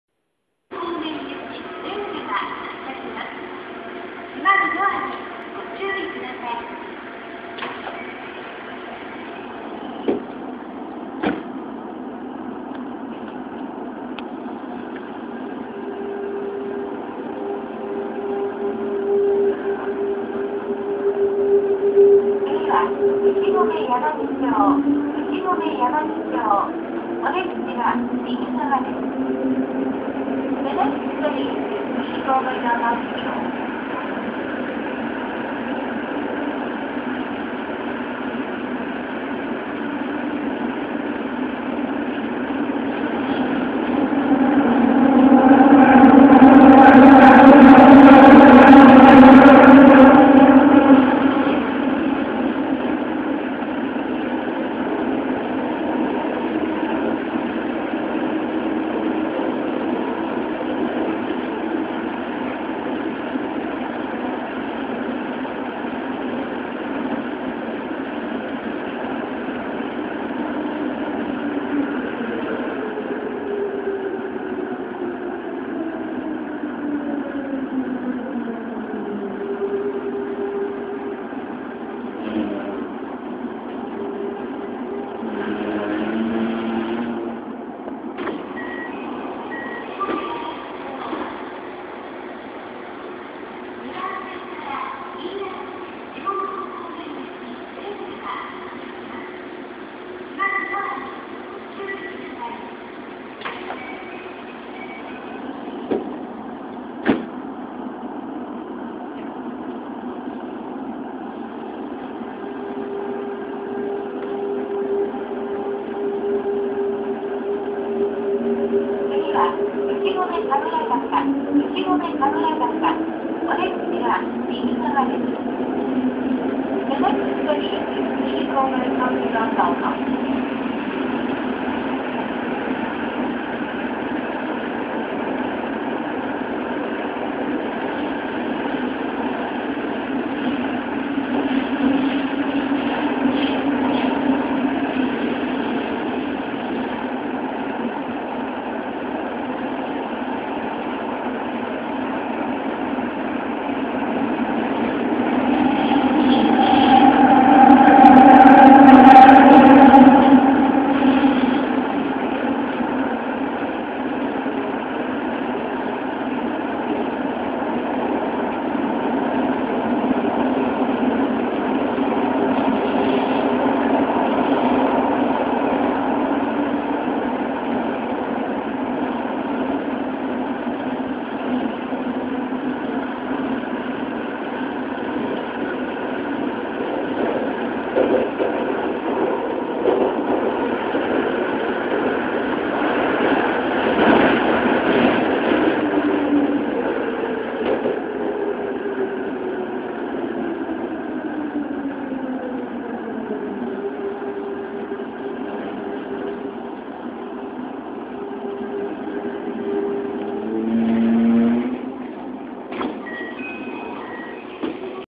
都営大江戸線12-000形初期車走行音 若松河田～牛込神楽坂
都営大江戸線12-000形初期車(第1～6編成)の若松河田(E-03)→牛込柳町(E-04)→牛込神楽坂(E-05)間の走行音です。
制御方式は日立製T-INV12形のGTOサイリスタ素子VVVF制御です。乗ったことはないのですが、磁励音は大阪市交70系と酷似しています。

車両：都営12-000形　第6編成2次車　12-065F